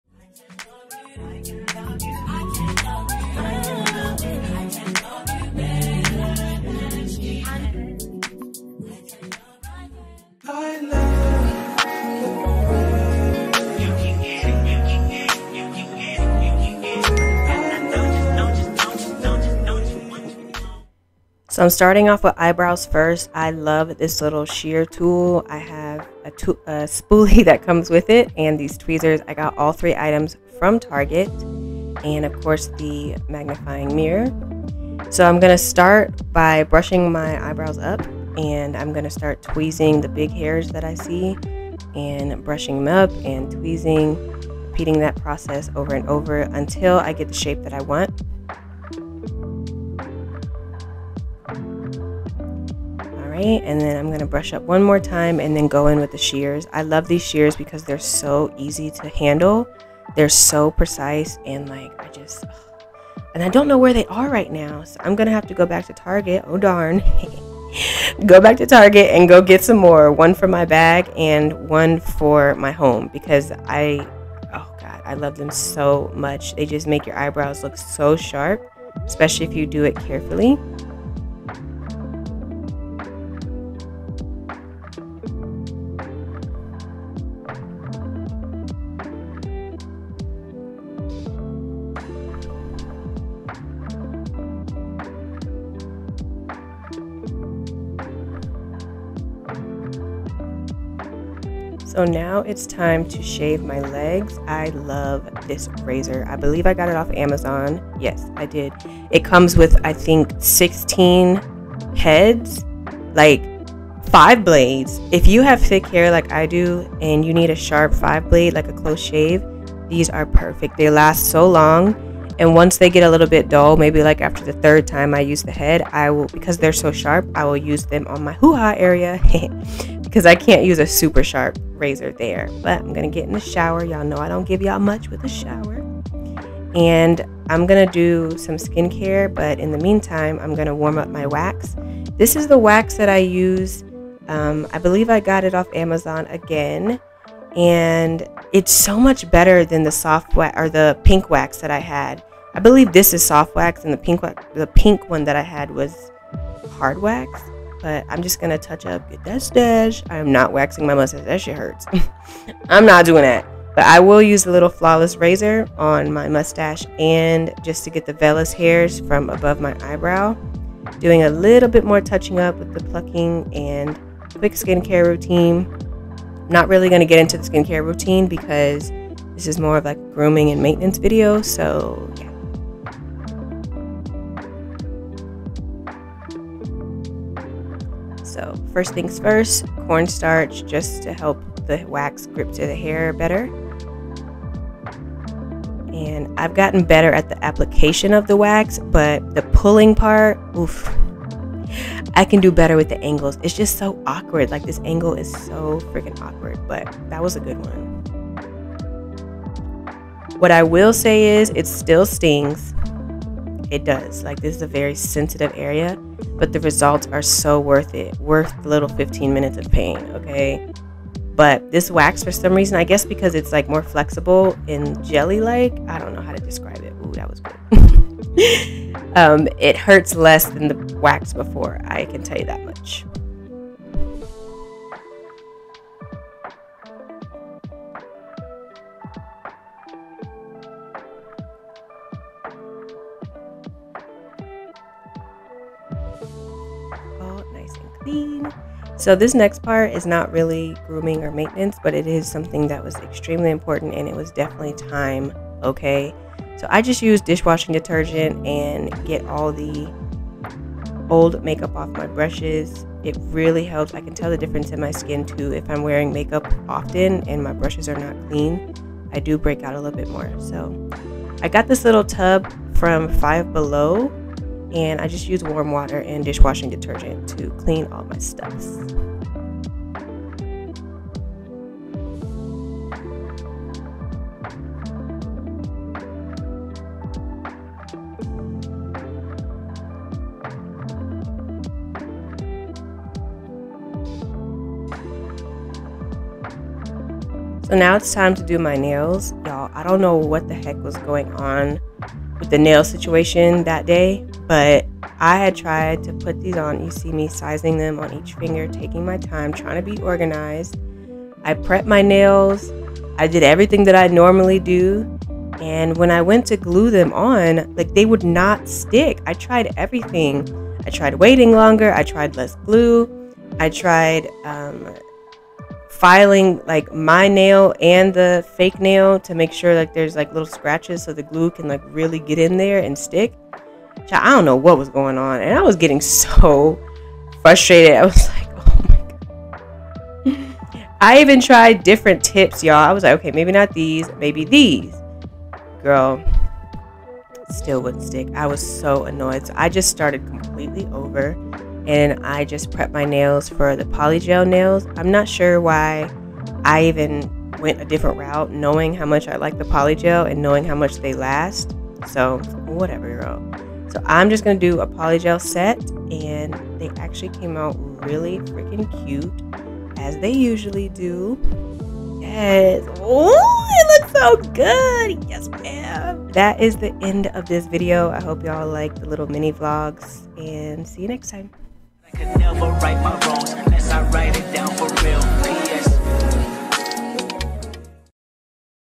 SELF CARE ROUTINE | GROOMING & MAINTENANCE | WAX. SHAVE. SKIN CARE. NAILS. | PAMPER ROUTINES
HEY Y'ALL🫶🏽
WELCOME TO A SERIES OF MINI VLOGS THAT INCLUDE DIFFERENT THINGS THAT I LIKE TO INDULGE IN FOR SELF CARE/PAMPERING. INSTEAD OF POSTING ONE BIG VIDEO WITH EVERYTHING I ENJOY, I DECIDED THAT I'M GOING TO DO MINI SESSIONS HERE AND THERE. IT'S SO MUCH MORE REALISTIC AND I DIDN'T WANT TO OVERWHELM MYSELF WITH A FULL DAY OF TASKS. I'VE REALIZED DOING SMALLER THINGS HERE AND THERE WORKS BETTER FOR MY SCHEDULE AND IT GIVES ME SOMETHING TO LOOK FORWARD TO THROUGHOUT THE WEEK.

THIS SEASON CAN BE CHALLENGING FOR MANY OF US AND I ALWAYS WANT YOU TO REMEMBER YOU'RE NOT ALONE! SO GRAB YOUR FAVORITE DRINK, BOOK, FACE MASK, TV SHOW, BLANKET AND LOVE ON YOURSELF A LITTLE MORE DURING THIS TIME!

TWEEZERS: TARGET

BROW SCISSORS: TARGET/AMAZON
(I COULDN’T FIND THEM ON THE TARGET APP, BUT I FOUND SOME ON AMAZON)

WAX WARMER: AMAZON

BLUE WAX: AMAZON

RAZOR: AMAZON

FLAWLESS FACIAL RAZOR: AMAZON

POLYGEL KIT: AMAZON
KIT:
GEL COLOR USED IN VIDEO:

✨VLOGS POSTED TUESDAYS✨